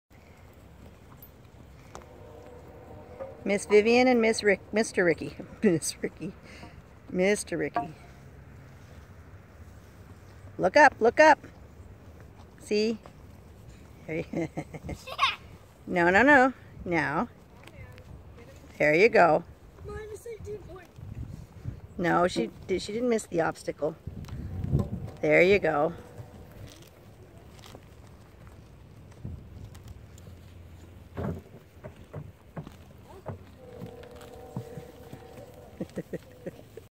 Vivienne and Ricky In Hand Mountain Trail
In Hand. 6 year old Vivienne with Ricky, Purebred Arabian Gelding Practicing on our very crude trail course.  But having fun.